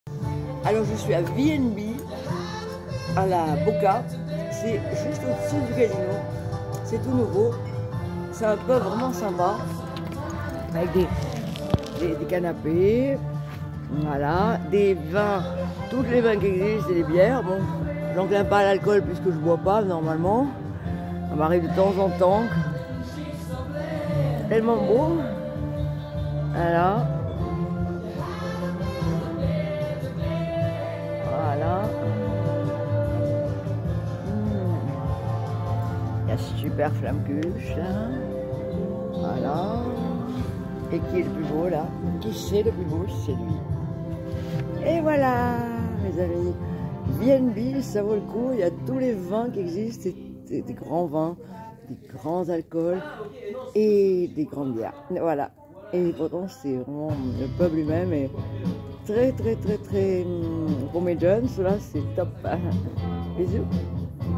@V&B cannes UN PUB PAS COMME LES AUTRES AVEC @JonathaneFly26 ET Lili Marley son bébé shiwawa 🐱
le V&B est un immense cave à vin avec les plus grands alcools et un choix de bières immense est à côté de cela il y a le pub qui est l'endroit où Lily Marley fait sa star et où il revit car toutes les tables lui envoie la balle et je suis dû la salle entière avec son charme et sa façon de faire irrésistible je ne vais au pub quasiment que pour lui même si je trouve l'endroit formidable avec des gens très sympathiques un accueil exceptionnel un cadre formidable sans parler de la délicieuse flammekueche et en plus ils font des manifestations formidables avec des orchestres ou des blind test je vous le conseille vivement si vous voulez un moment de détente en famille, entre amis ou même seul bisous à toute l'équipe qui est formidable